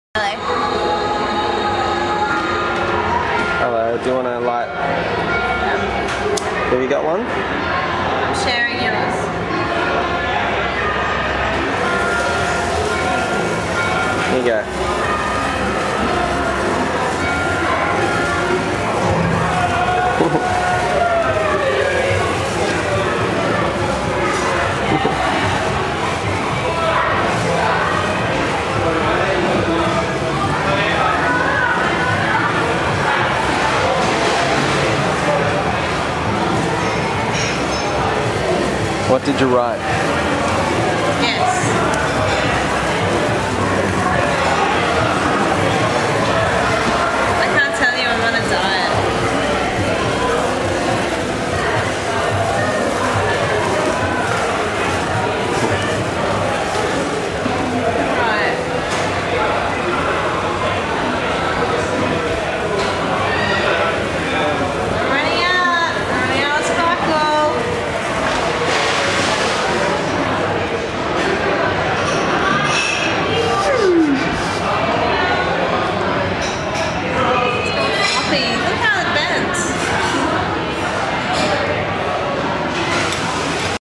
Ocean Sparkle
Sparkler at Fat Boys the day after my birthday